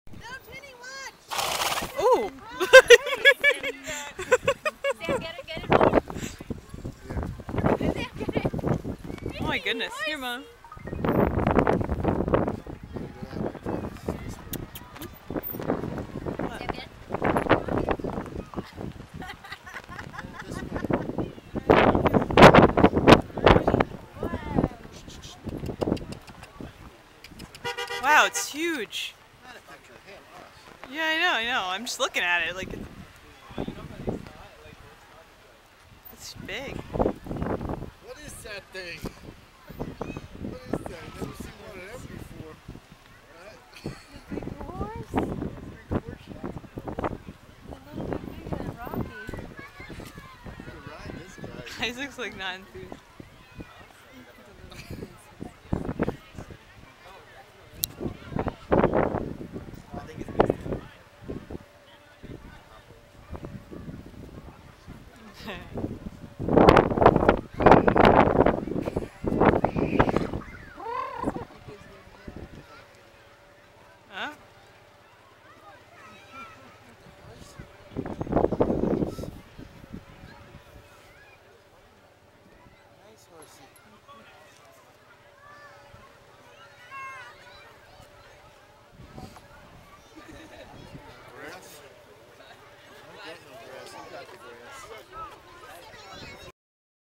Horse Upstate
Isaac's first encounter with a horse... at the beginning the horse spit stuff on me lol!